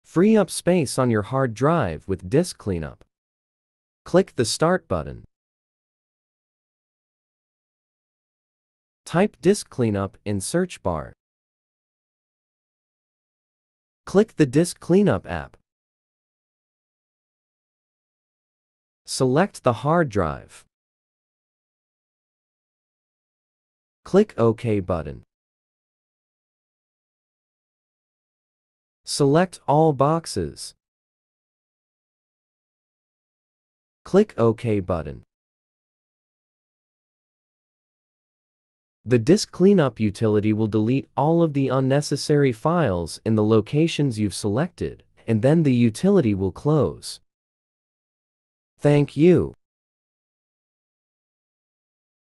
Free up space on your hard drive with Disk Cleanup
How to Clean Your Hard Disk# With Disk

Select the Start button and type Disk Cleanup. Select the Disk Cleanup app. When it launches, you'll see all of the files on your system and how much space clearing them would make available.

The Disk Cleanup utility will delete all of the unnecessary files in the locations you've selected, and then the utility will close.